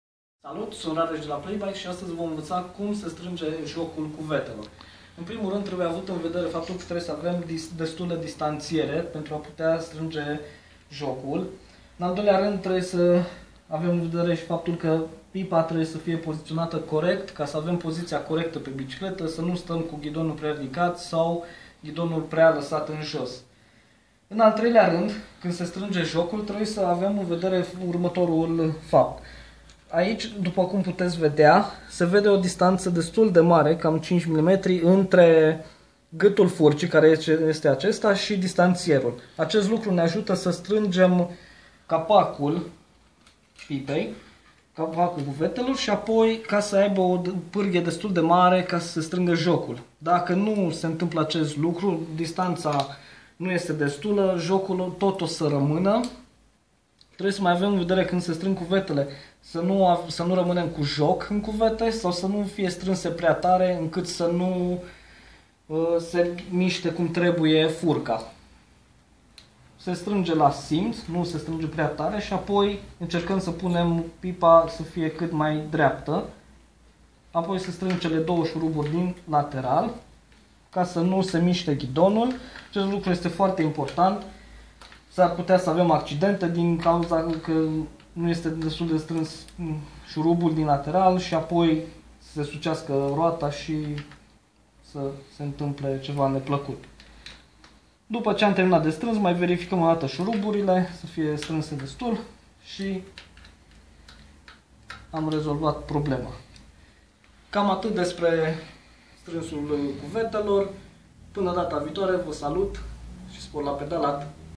PlayBike te ajută - Cum strângem cuvetele? (ep. 11)
PlayBike TE AJUTĂ! :)

Aceasta este o serie de filmulețe prin care încercăm să le explicăm tuturor începătorilor cum să rezolve singuri câteva operațiuni elementare care țin de montarea și de întreținerea bicicletelor lor.

Cu jocul nu-i de joacă! O operațiune importantă, dar simplu de rezolvat, ilustrată aici de Rareș, pentru a lua direcția cea bună în siguranță.